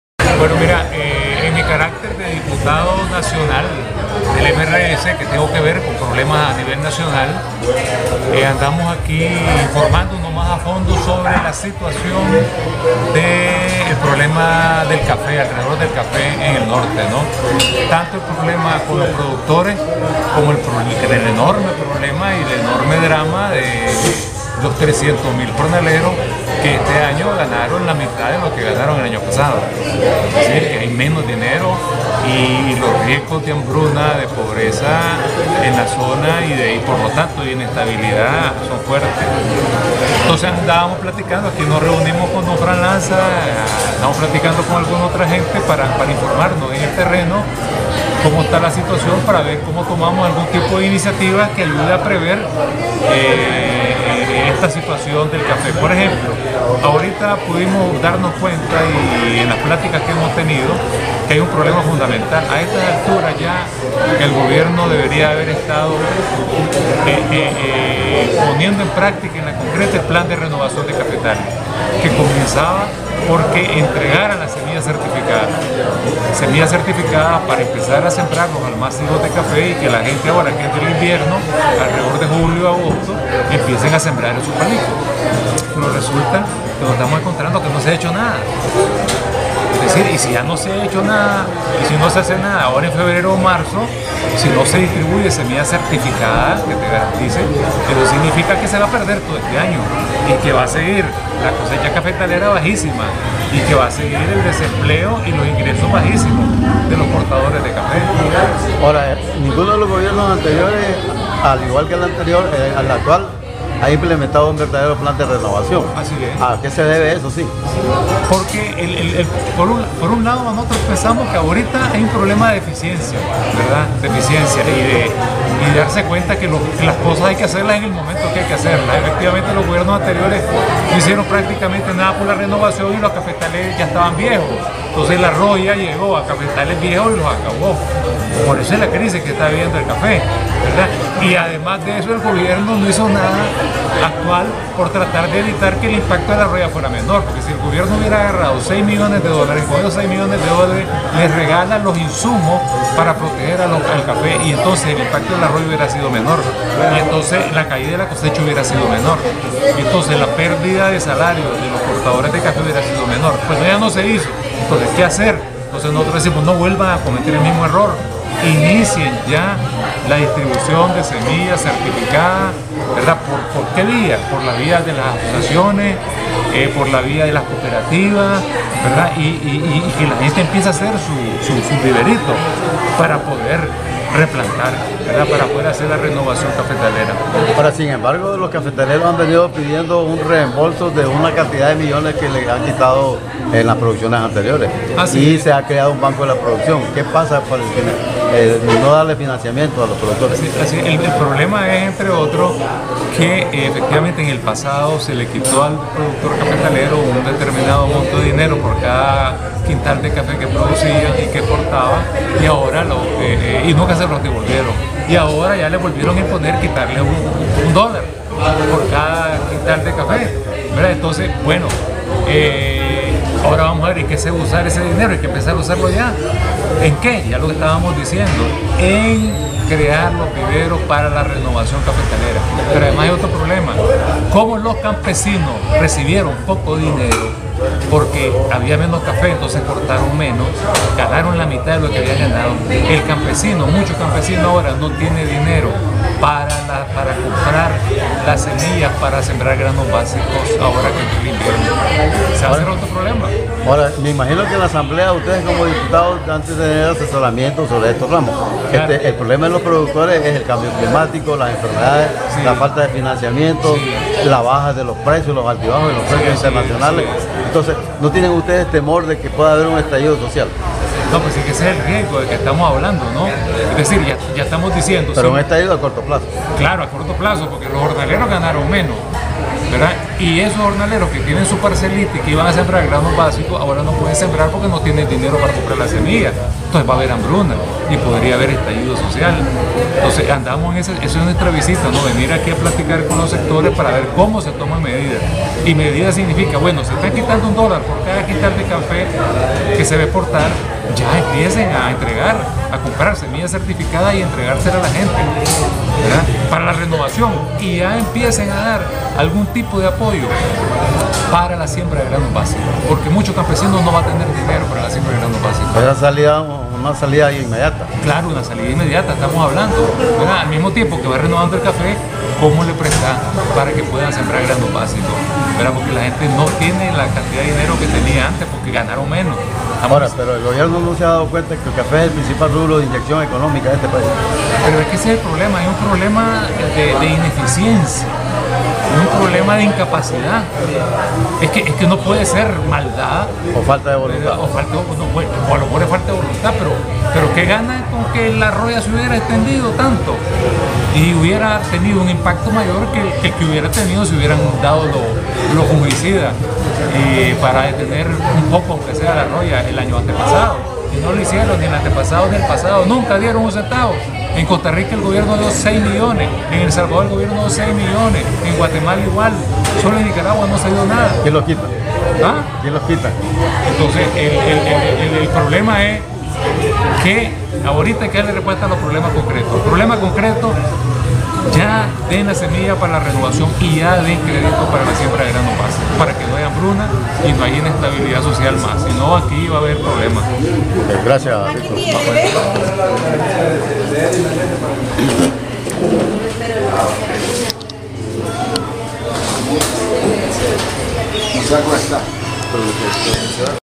Victor Hugo Tinoco, diputado nacional del MRS
Victor Hugo Tinoco, diputado nacional por el MRS, señala que si el gobierno no da una repuesta de habilitaciòn a corto plazo a los caficultores, podrìa darse un estallido social, y tambièn considera que el financiamiento debe de ser para los productores de granos bàsicos antes que sea tarde. "Es triste ver como pequeños productores de cafè se convirtieron en cortadores y por falta de voluntad el gobierno que tadavía no hace nada". Señala el parlamentario que en otros paises centroamericanos se han facilitado hasta sesi millones de dolares al rubro cafè y en Nicaragua, es al contrario, ya los cafetaleros han comenzado a dar un dòlar por quintal exportado y no reciben nada del gobierno. Más detalles sobre el tema en la entrevista dada por el parlamentario Victor Hugo en Matagalpa, luego de haberse entrevistado con varios ex directivos de la Asociaciòn de Café de Matagalpa, ASOCAFEMAT.